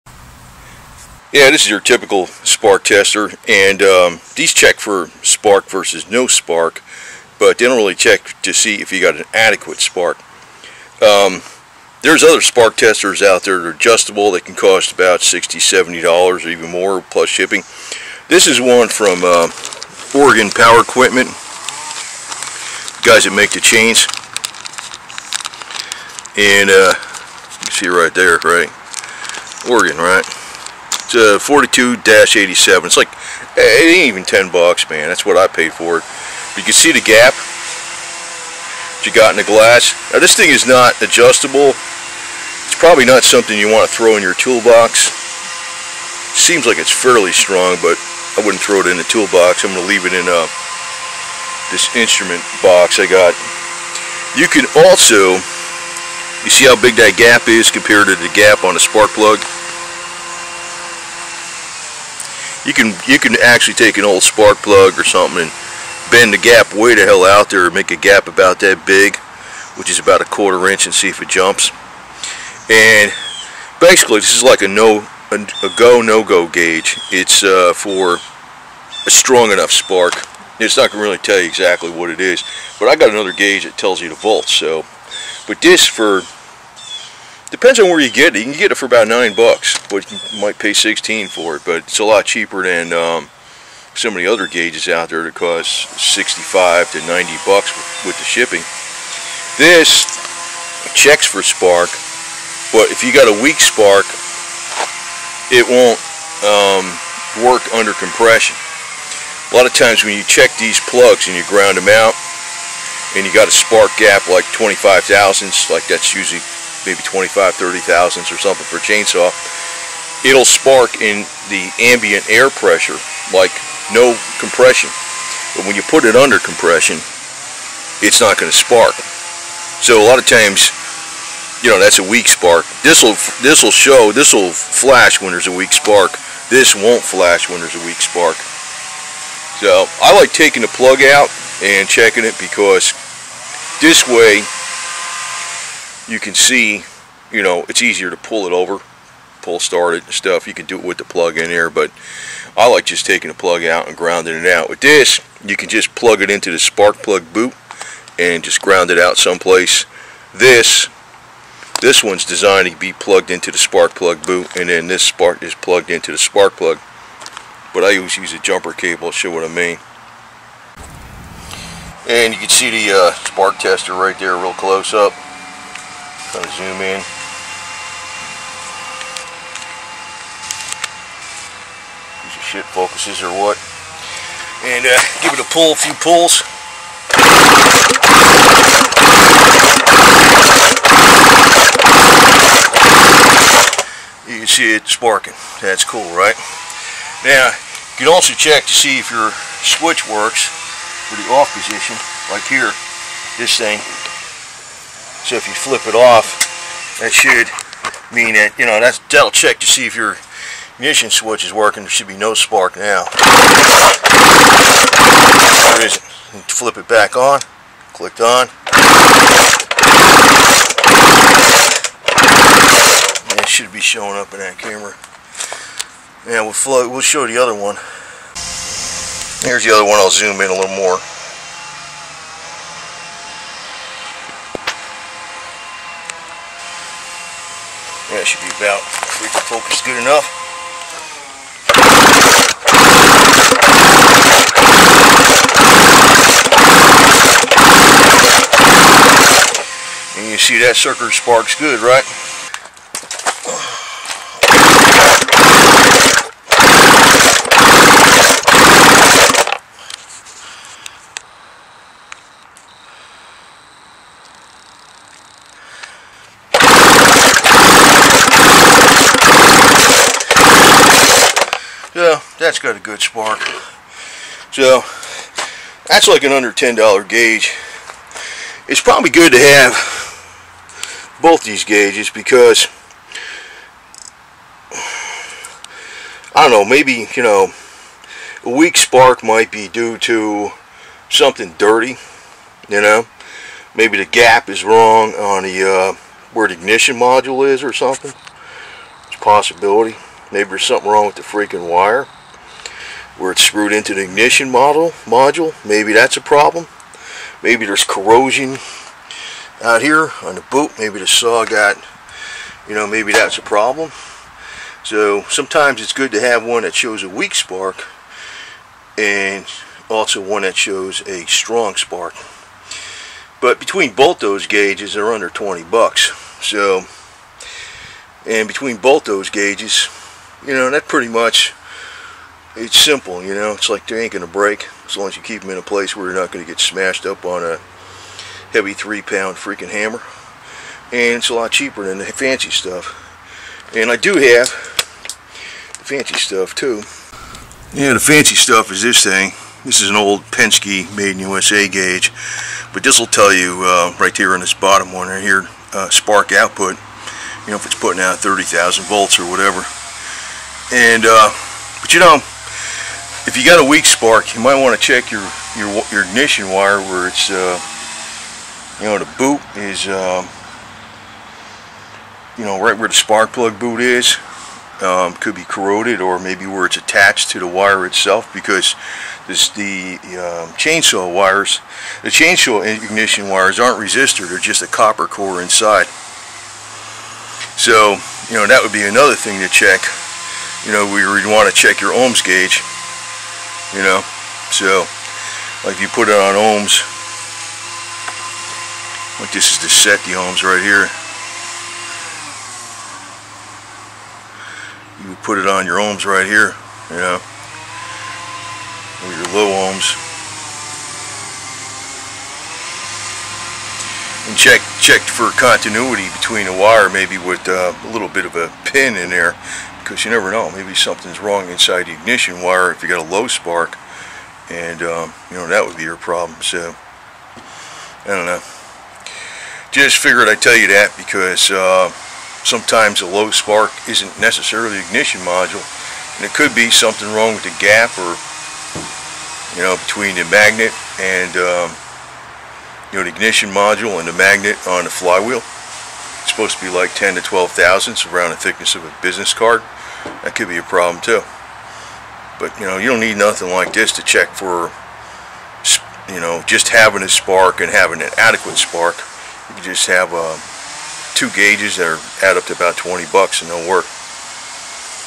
Spark Tester vs a STRONG Spark Tester
Some spark testers won't tell you if you have a weak spark.  You can have a visible spark, but under compression that spark won't be strong enough to ignite the fuel/air mixture.  What you need is a spark tester that tests for a strong spark.  Both these types of spark testers are of the go / no-go type and it's good to have both types of spark testers.   I like to check for spark without using the spark plug and using a ground wire so you can eliminate a problem with the spark plug.  If you show a strong spark, test it again with the spark plug as the ground and if it shows spark, then you know the ignition module, spark plug wire and spark plug are all good.  If you show spark with the cheaper spark plug tester and no spark with the strong spark plug tester, it might be a problem in the ignition wire or the gap setting of the ignition module to flywheel.  In this case check and adjust the ignition module gap and then use an ohm meter to check the spark plug wire.  The spark plug wire used on a chainsaw has a solid copper core and isn't a resistor type ignition wire so an ohm meter should show good continuity to the end where spark plug boot is.  You might not need an ignition module in the case of a weak spark.